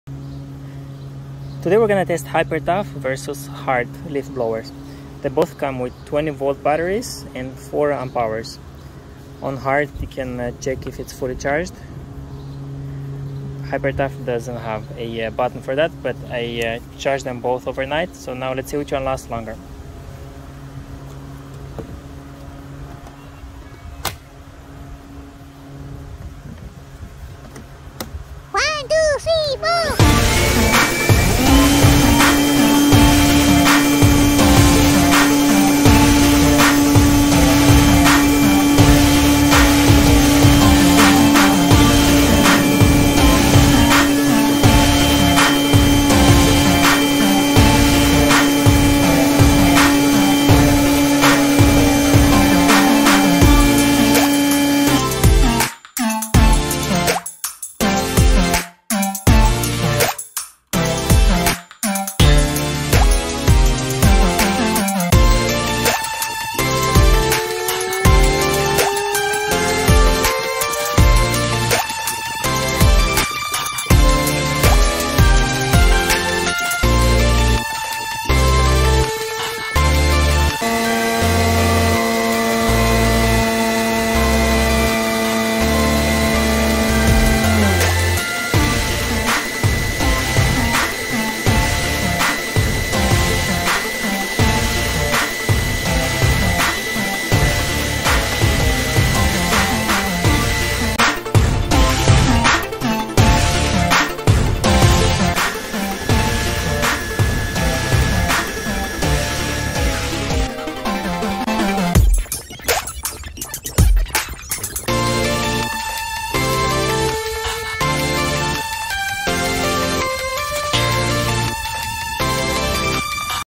Walmart leaf blowers: Hyper Tough vs Hart duration test
hyper tough HT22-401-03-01 $107 420 CFM 110 MPH brushless
hart comes in a combo with a 13in string trimmer for $158 at 315 CFM 100 MPH brushed